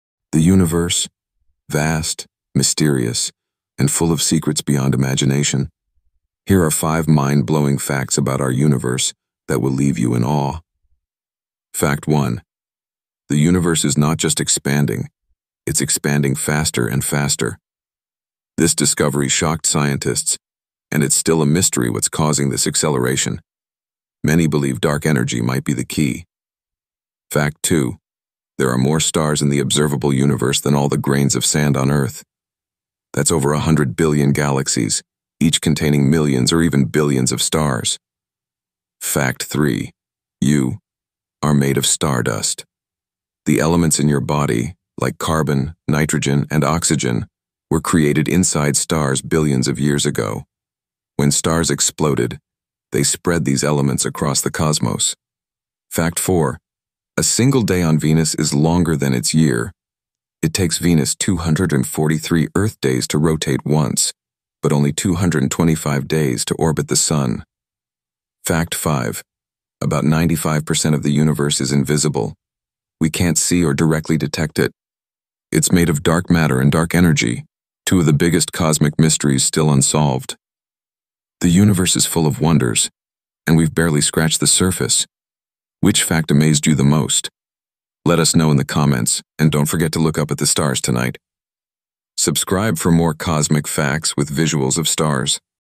5 Mind-Blowing Facts About the Universe You Didn't Know!
5 Mind-Blowing Facts About the Universe You Didn't Know!

Discover the most amazing and mysterious facts about our universe!
In this short video, you'll learn 5 incredible truths about space that will blow your mind – from the stars being older than you can imagine to secrets of dark energy. Whether you're a space lover or just curious about the cosmos, this video is for you!

Chapters:
0:00 - Intro
0:15 - The Universe is Expanding Faster
0:40 - Stars vs Grains of Sand
1:05 - You Are Made of Stardust
1:30 - A Day on Venus is Longer Than a Year
1:55 - 95% of the Universe is Invisible
2:20 - Outro